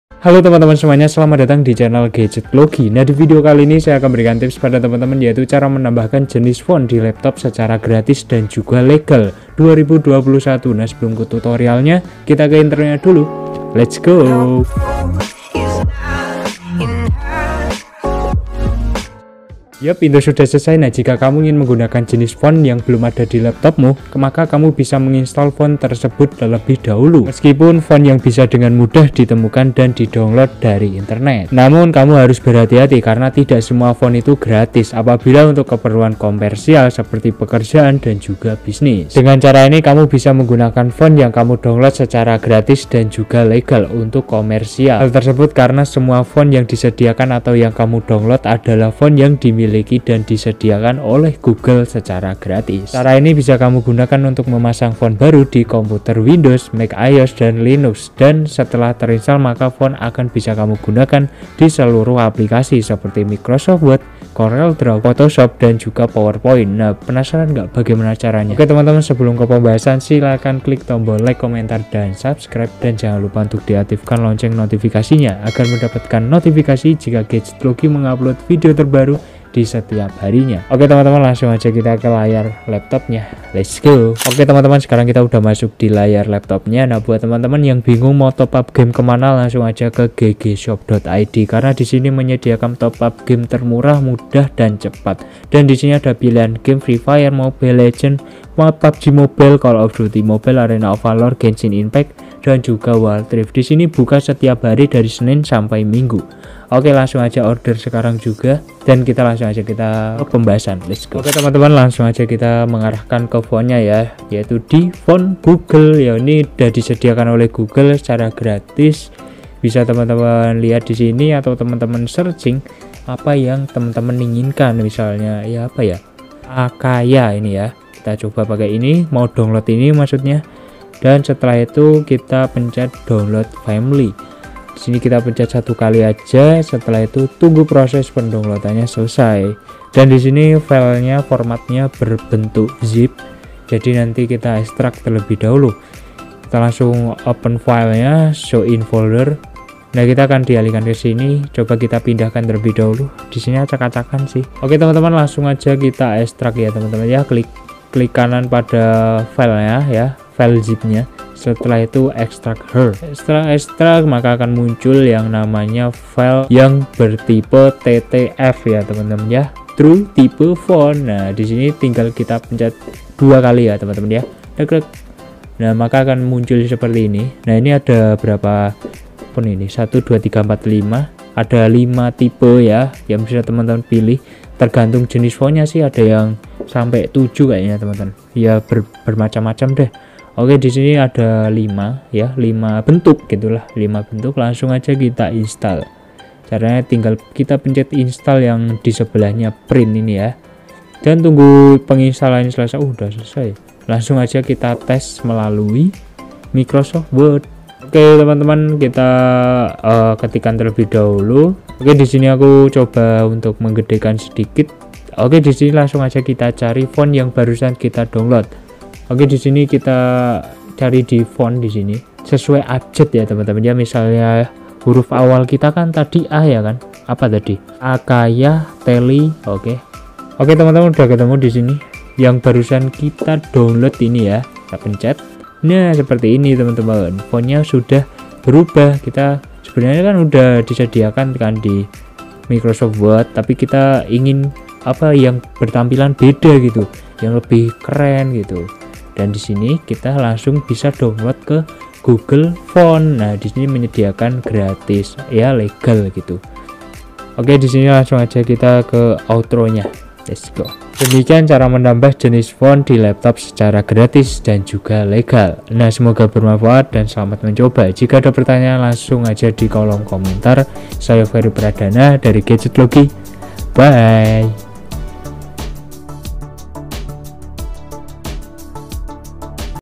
CARA MENAMBAH JENIS FONT DI LAPTOP SECARA GRATIS DAN LEGAL (2022)
🔥 Voucher Gratis Ongkir Shopee Hari Ini 🔥

Jika kamu ingin menggunakan jenis font yang belum ada di laptopmu, maka kamu bisa menginstal font tersebut terlebih dahulu.

Meskipun font bisa dengan mudah ditemukan dan didownload dari internet, namun kamu harus berhati-hati karena tidak semua font bisa digunakan secara gratis, apalagi untuk keperluan komersial seperti pekerjaan dan bisnis.

Dengan cara ini, kamu bisa menggunakan font yang kamu download secara gratis dan legal untuk komersial. Hal tersebut karena semua font yang kamu download adalah font yang dimiliki dan disediakan oleh Google secara gratis.

Cara ini bisa kamu gunakan untuk memasang font baru di komputer Windows, MacOS dan Linux dan setelah terinstal maka font akan bisa digunakan di seluruh aplikasi seperti Microsoft Word, Corel, Photoshop dan PowerPoint.

Ingin tahu bagaimana caranya? silahkan simak video ini sampai selesai.